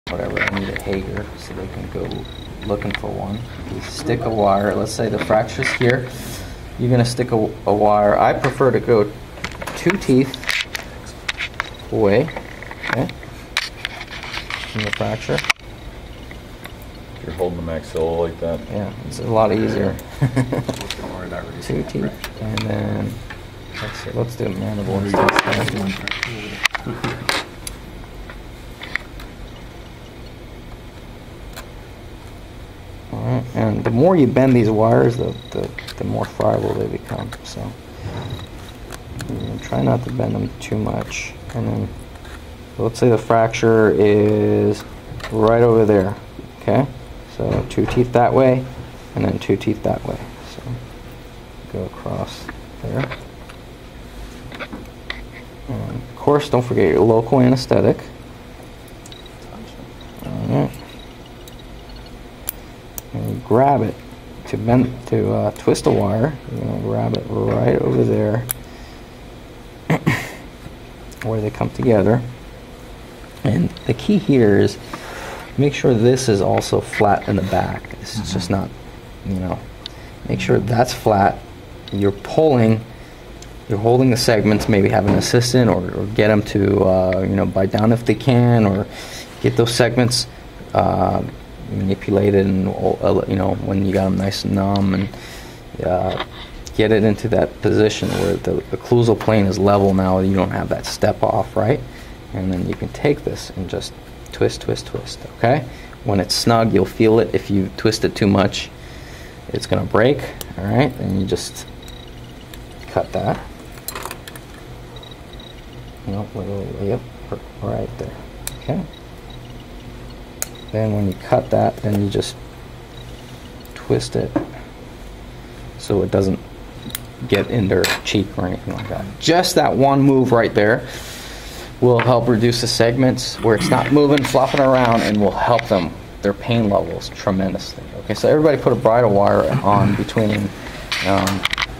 Jaw Fracture - Bridal Wire placement
For closed reduction techniques (ie wiring jaw shut), one of the key wires to place is a bridal wire.  This will serve to stabilize the fractured mandible before MMF, but also relief a signifiant amount of pain - why?